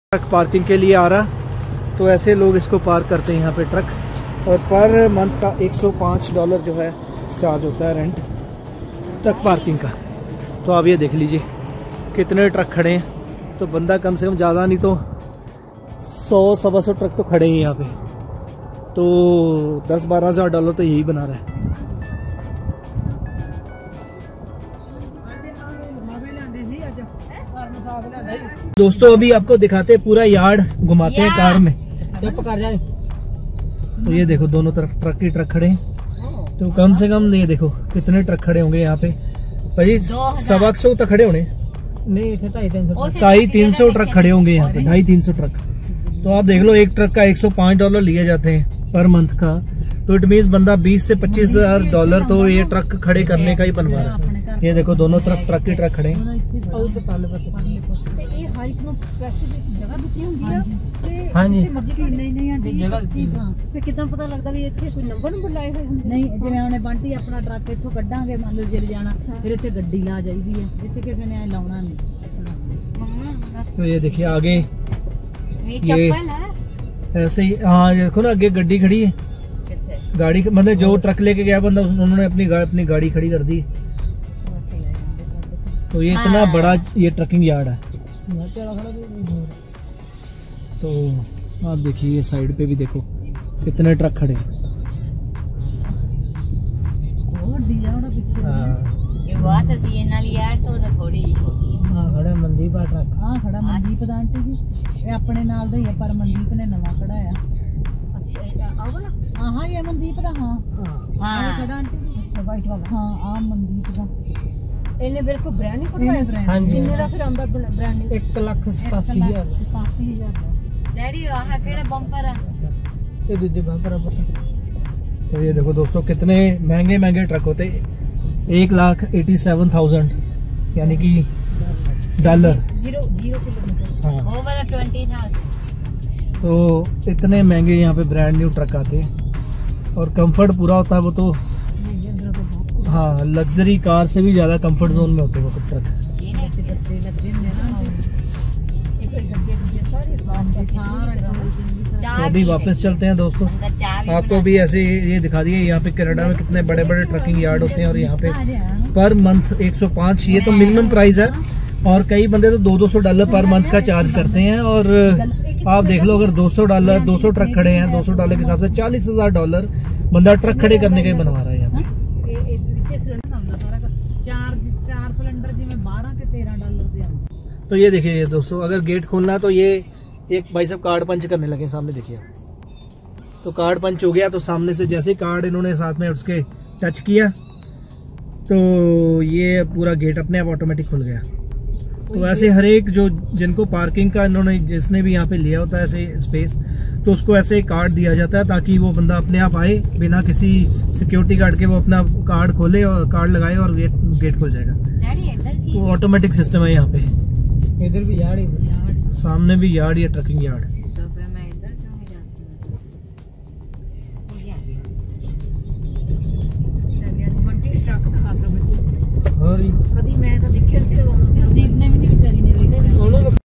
Truck Yard In Canada, Calgary, What Rent Paid For Parking, How Truck Yards Work For Canadian Drivers
See Truck yards in Calgary Canada, every month from $100 To $250 Rent Paid by truck drivers to park their trucks in the truck yards for parking, see how Canadian truck driver opening and using truck yards to park their trucks and how safe these truck yards are .